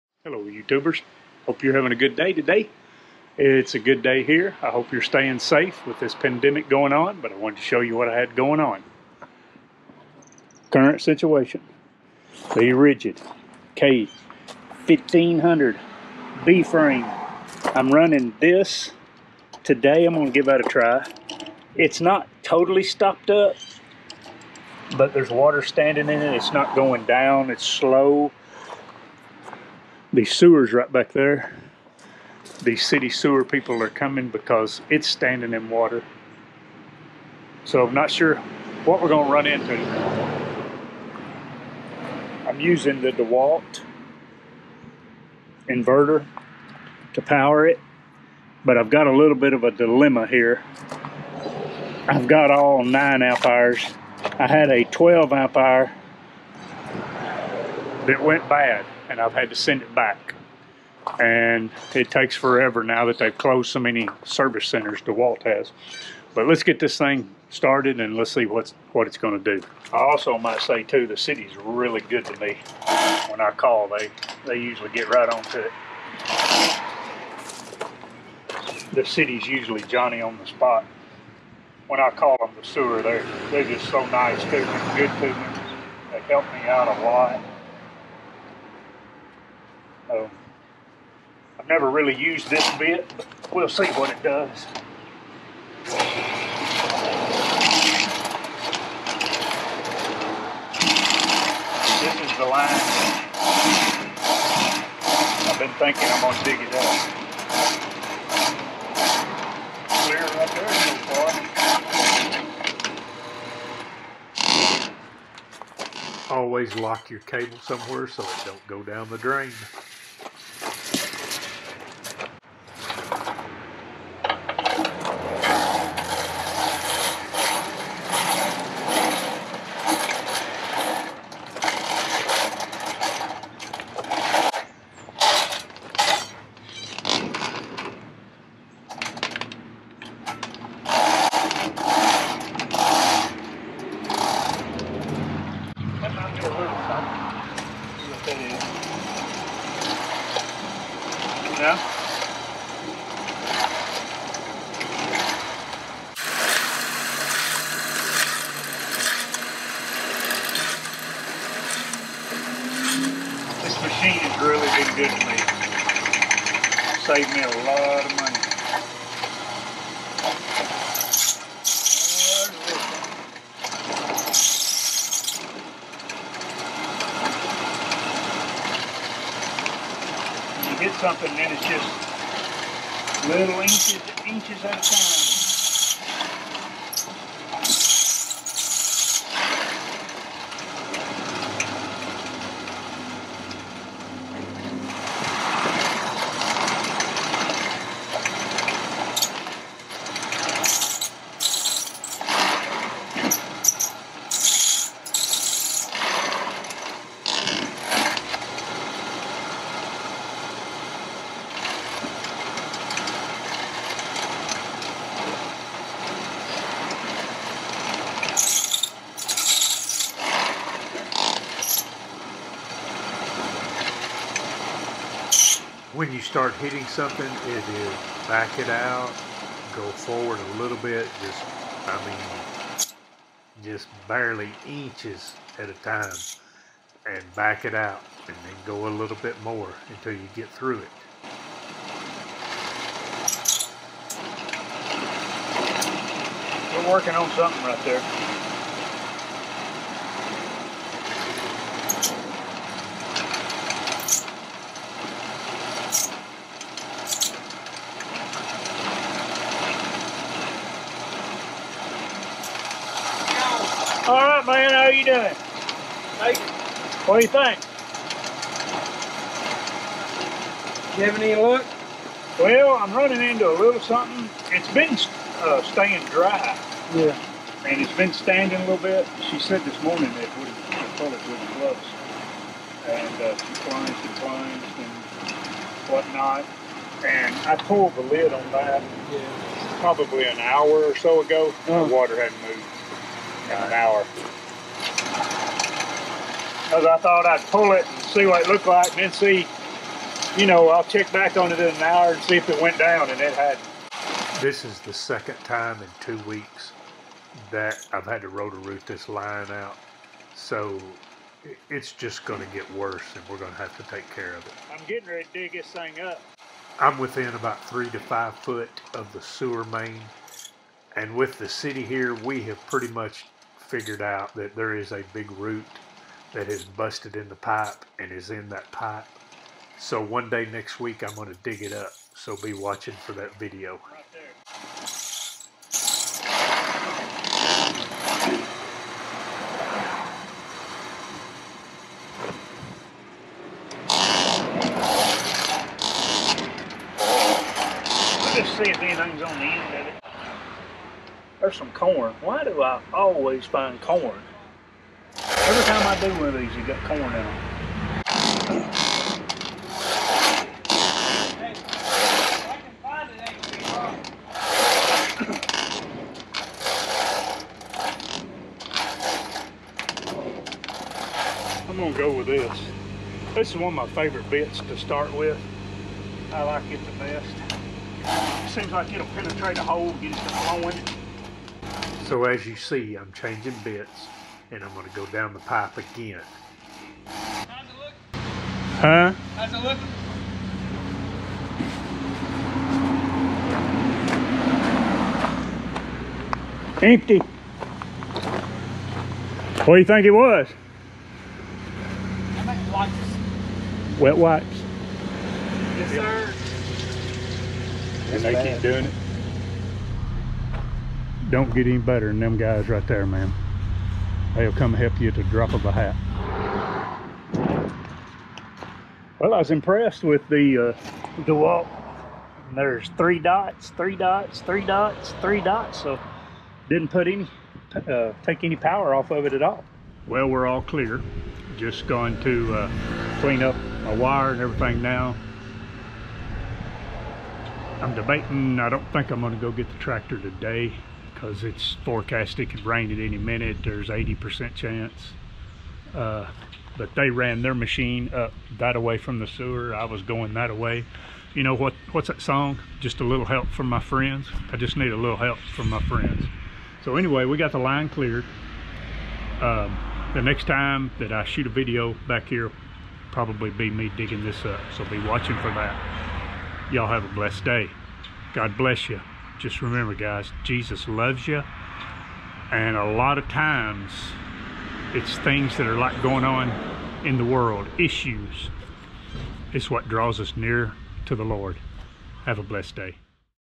Ridged finds corn again!!
Every time I use the Ridged K1500 I strike corn!! This Ridged K1500 sectional sewer cleaner has more than paid for it self. This machine is very helpful to me owning so many rental properties.

The city sewer guys are supper helpful.

filmed with GoPro 8 with Gopro mic